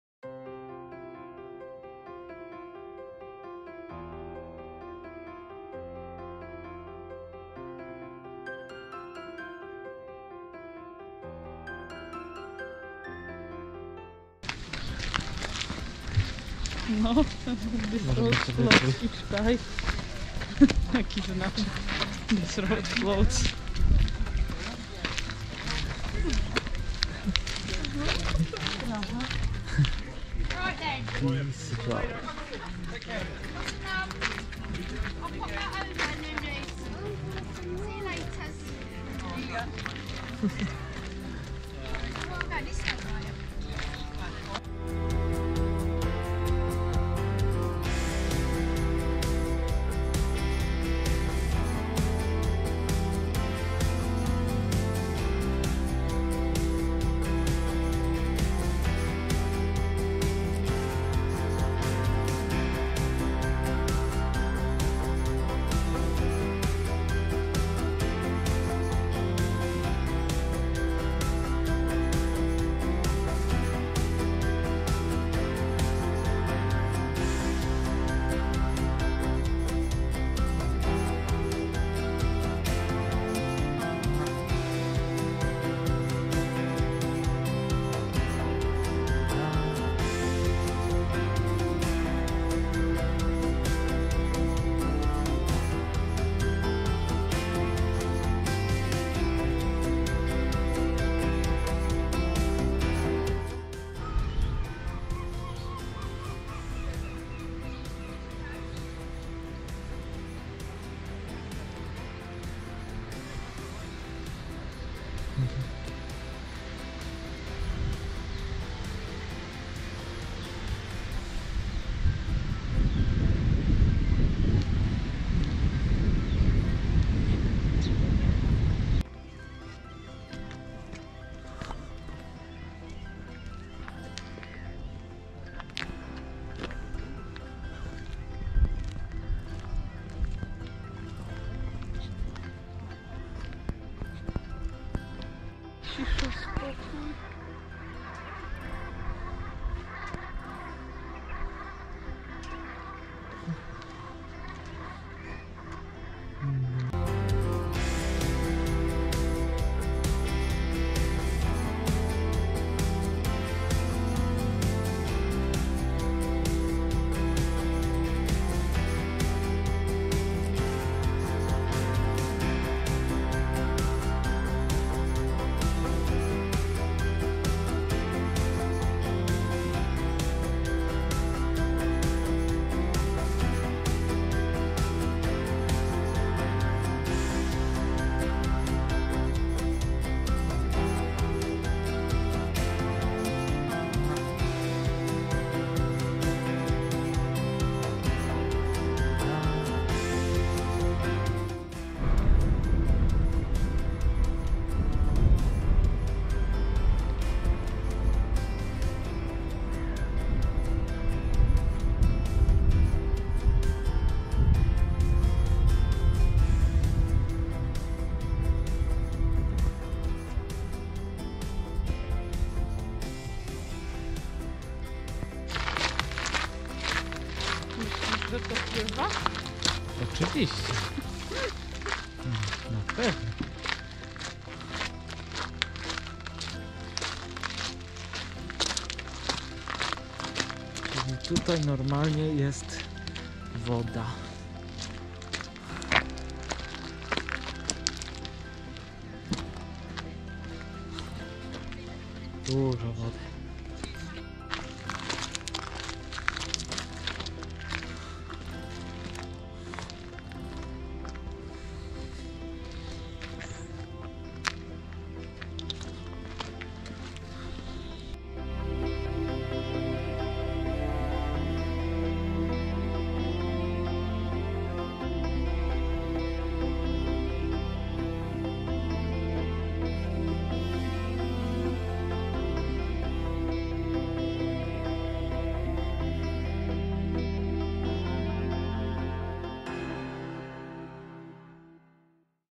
Pieszo przez Bosham, West Sussex
Krótka wypraa do misteczka Bosham które jest opisywane w angielskich przewodnikach jako "Angielska Wenecja", jest to oczywiście przesadzone stwierdzenie aczkolwiek podczas przypływów (dwa razy na dobę) zalewana jest ulica którą będziemy się przechadzać i podmywane są mury domów więc coś z Wenecji jest. Filmik został nagrany 7 marca. Życzę miłego oglądania i pozdrawiam